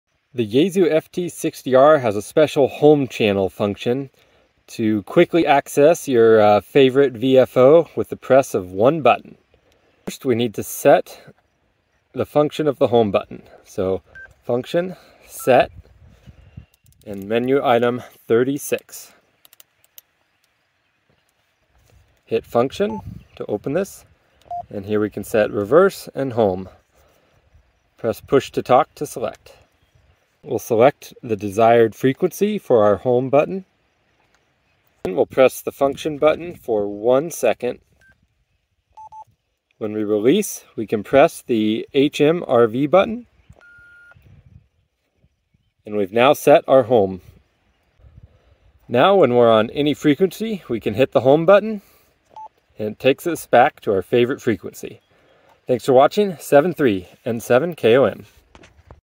How To Set the Home Frequency | Yaesu Ft-60r How To
How to set up a home frequency on thw Yaesu Ft-60r.

The Yaesu FT-60 is my favorite HT for Summits on the Air operations. In this video series, we're taking a deep dive to understand ALL aspects of this radio.